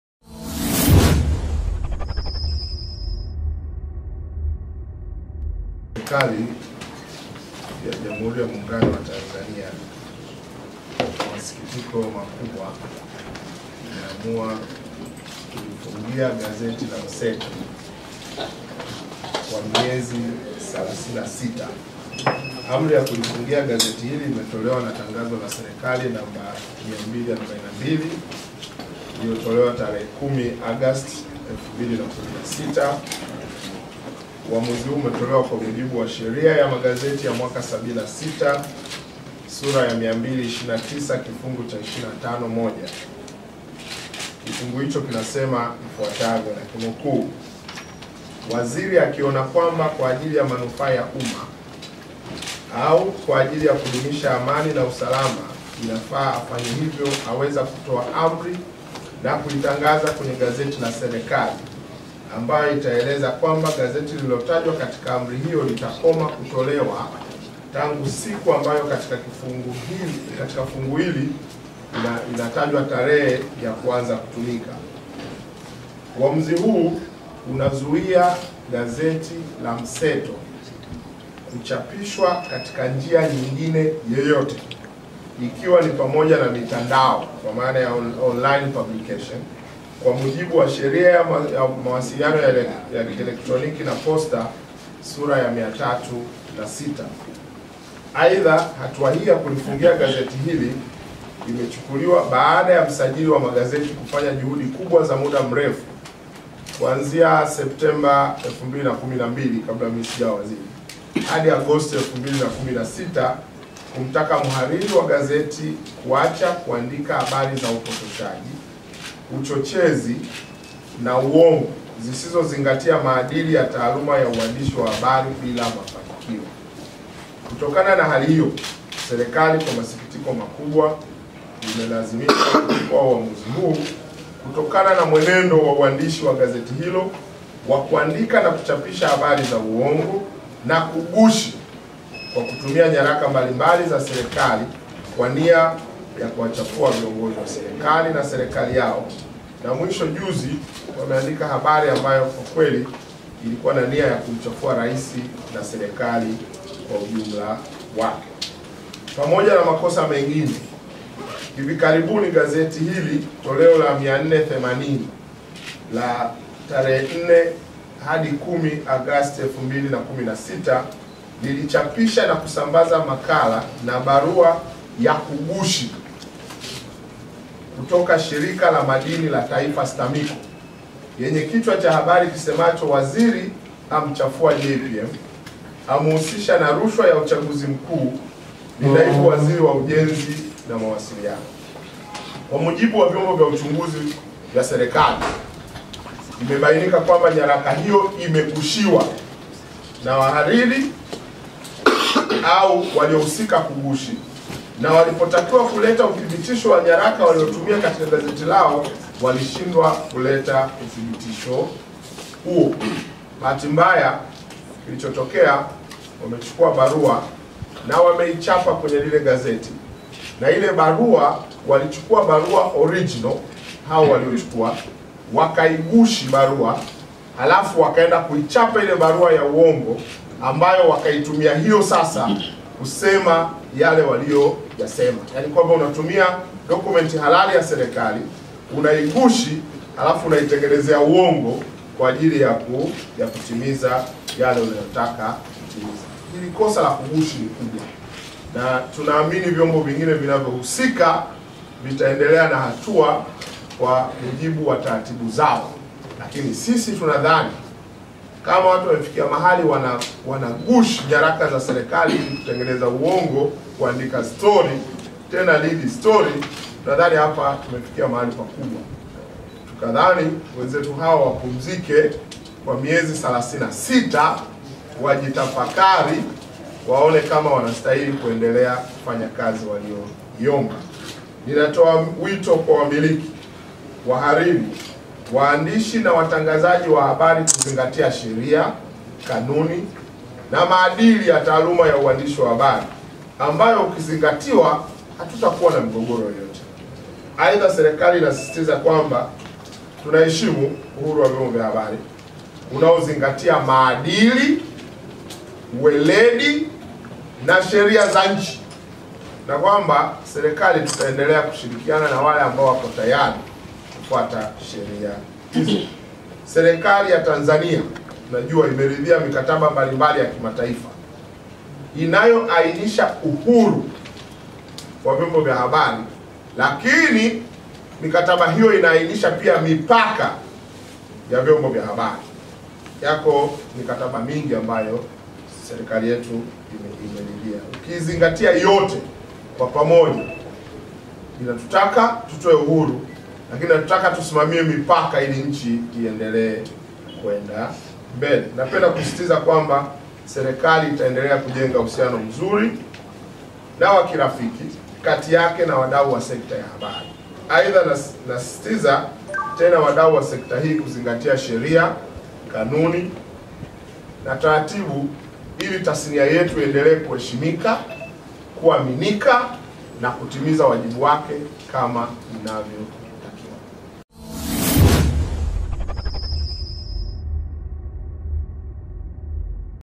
GAZETI LA MSETO LAFUNGIWA MIEZI 36
Waziri wa Habari, Utamaduni, Sanaa na Michezo, Nape Nnauye amelifungia gazeti la Mseto kwa miezi 36 kutokana na kuandika stori yenye nyaraka za kughushi.